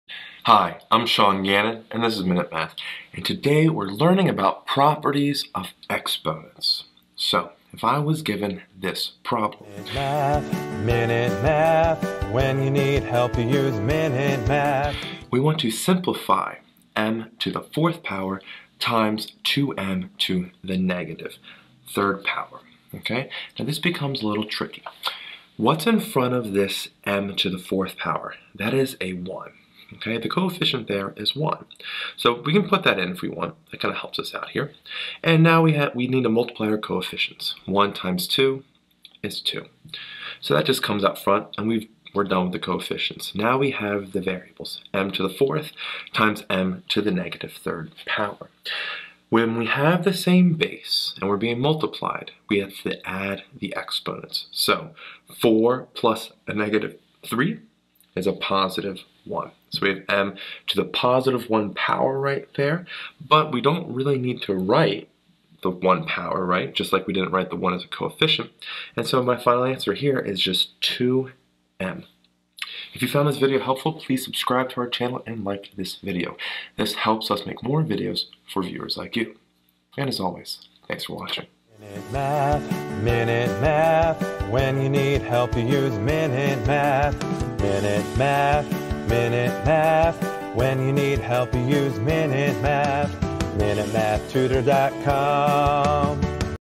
Simplify m^4*2m^-3
In this math video lesson we Simplify m^4*2m^ 3.